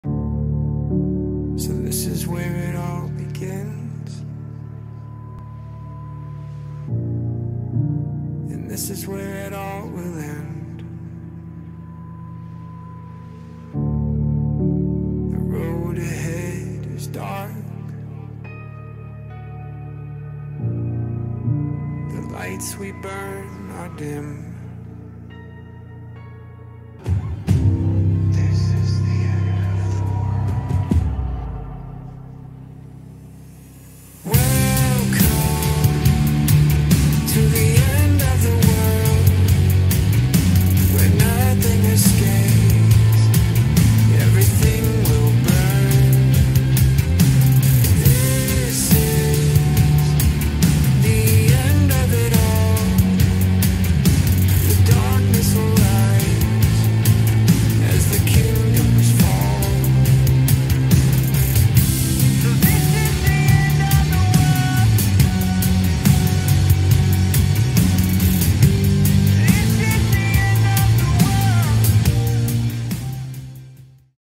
GHOUL-FORGE Neon LED Custom Plaque
Utilizing the Glowforge laser cutter, some LEDs, and a mix of wood and acrylic, we've put together an awesome plaque featuring one of the latest Lucky Needle / Skulls & Succulents designs!